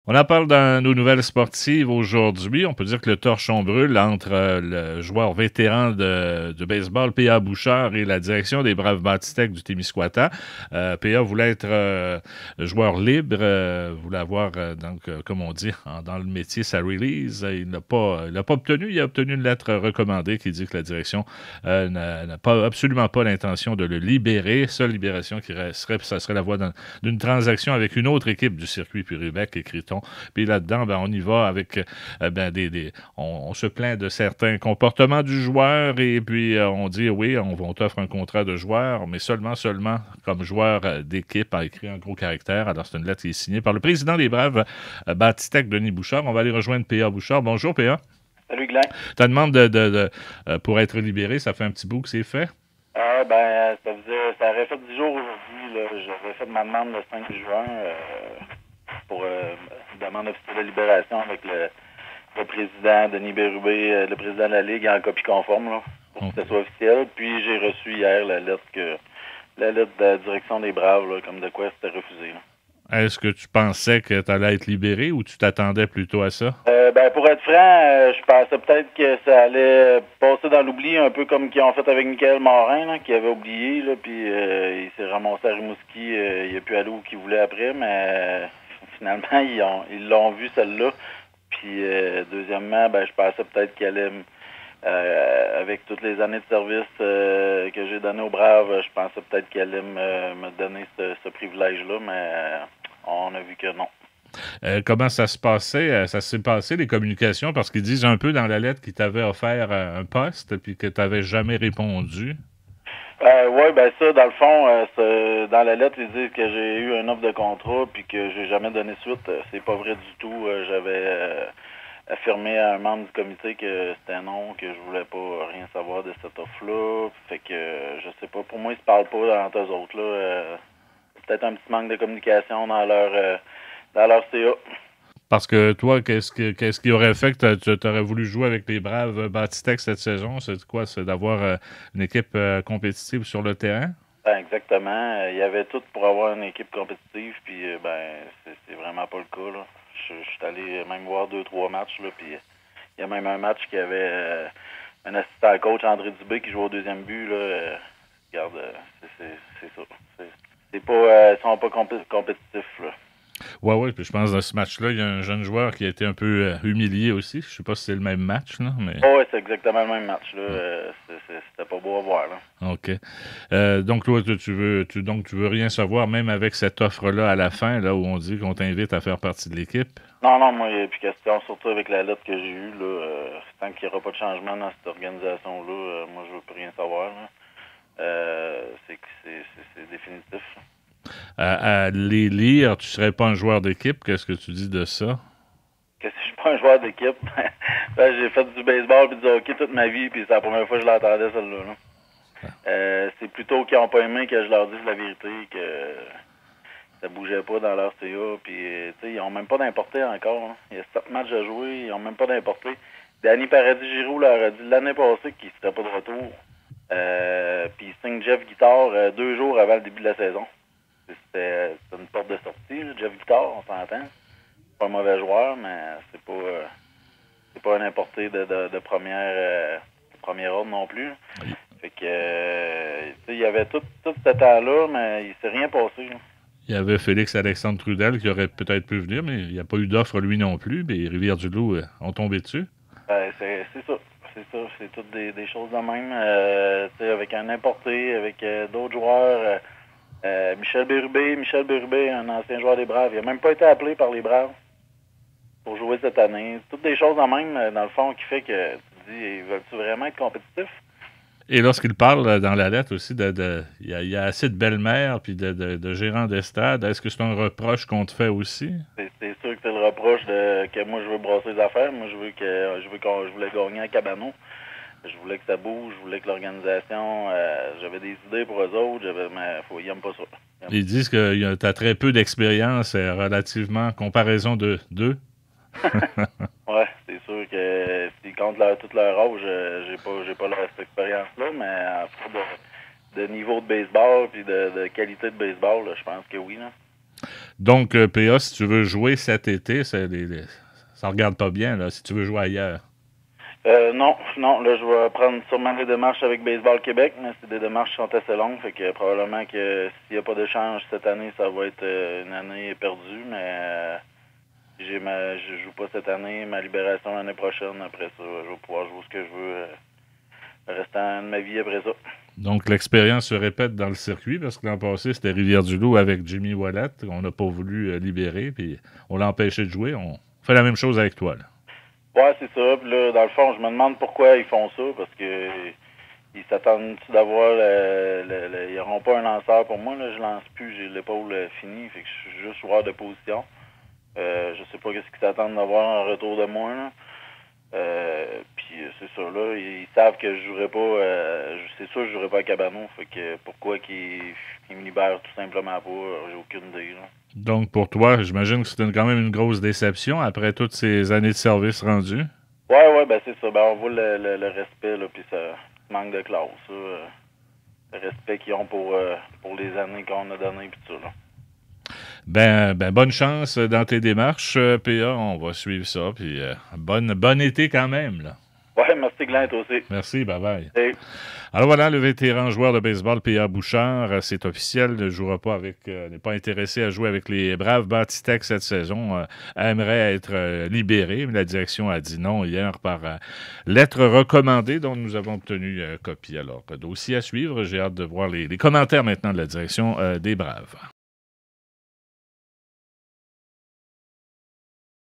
Entrevue avec P-a Bouchard qui accuse un refus pour sa libération des Braves Batitech. Info-Plus av
Entrevue avec P-a Bouchard qui accuse un refus pour sa libération des Braves Batitech. Info-Plus avec Guylain Jean, 19 Juin 2019